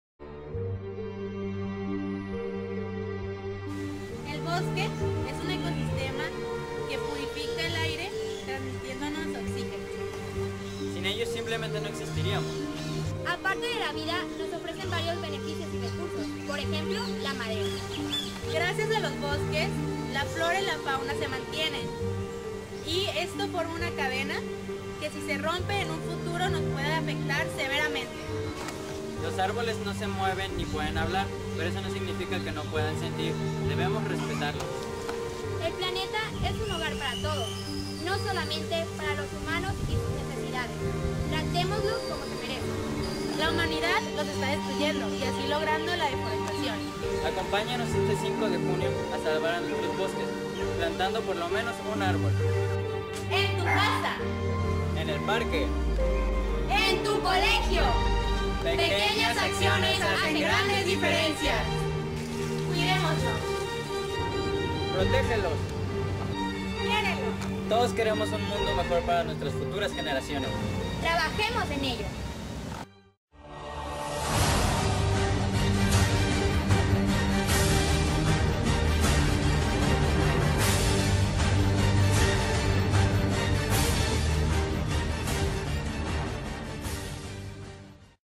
Dia Mundial Del Medio Ambiente
Un video en el cual se trata de apoyar al planeta promocionando a una organización que ayuda a cuidar la Tierra llamada UNEP.